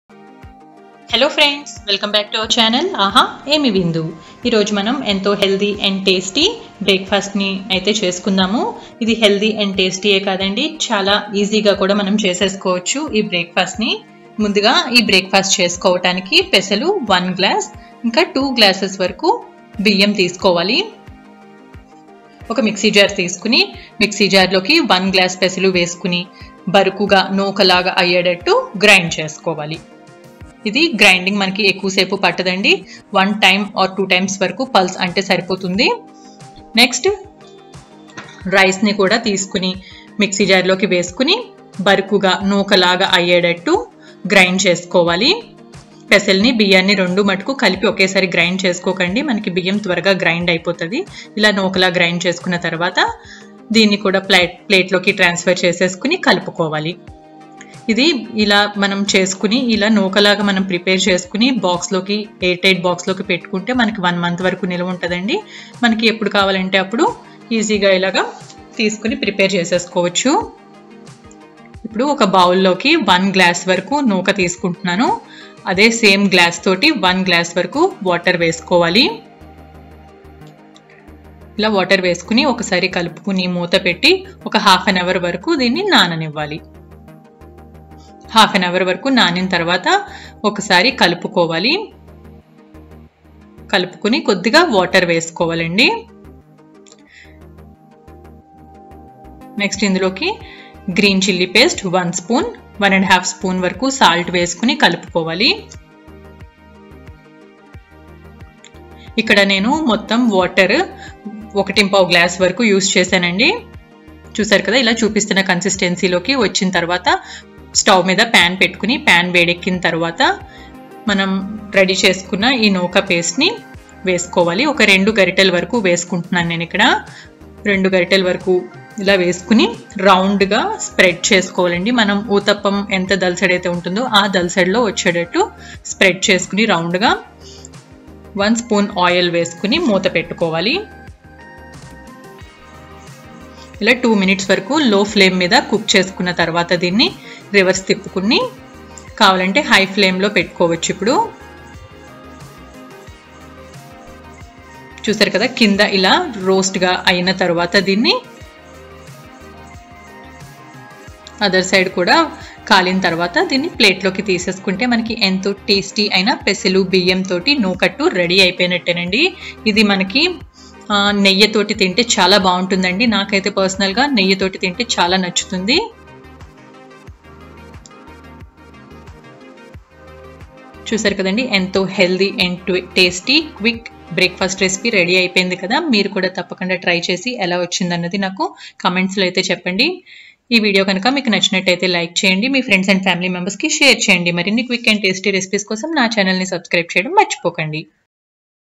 High Protein Pesalu Ravva Attu | Quick & Instant Breakfast Recipe | No Semolina & NO Maida recipe.
Hello Friends.. Welcome to Aha Emi Vindhu!!!
Today I am sharing, High protein Healthy & Quick Breakfast recipe with Moong Dal

Ingredients :
Rice : 2 cups
Moong Dal : 1 cup
Green Chilly Paste : 1 spoon
Salt : 1 1/2 spoon (as per taste)
Ghee
Water : 1 1/4th glass (as per consistency)

: Green peas rice with special masala

: Ginger pickle (మామిడికాయ అల్లం -వెల్లుల్లి నిల్వపచ్చడి)

Please share your opinions, it will help me to improve more.